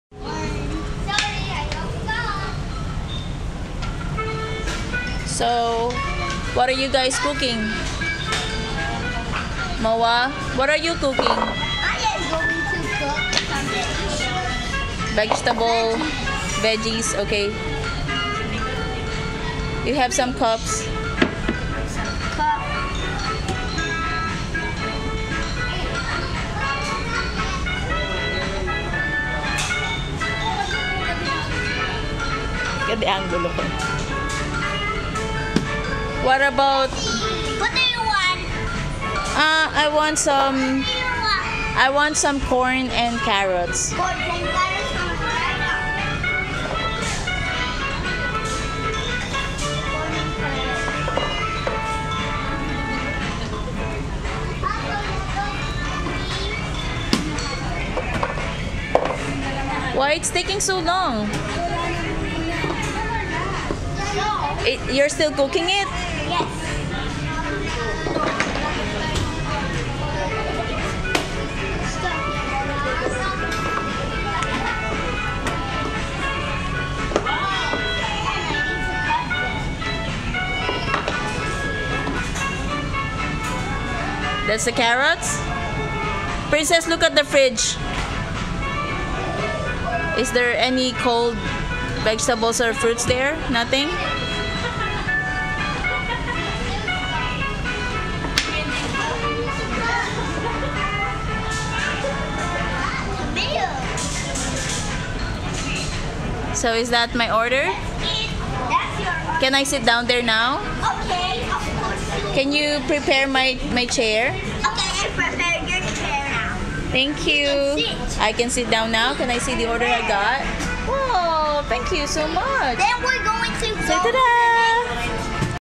Cooking on Noona's corner Part 2
So it is a mini canteen this time around as they ask me for my order..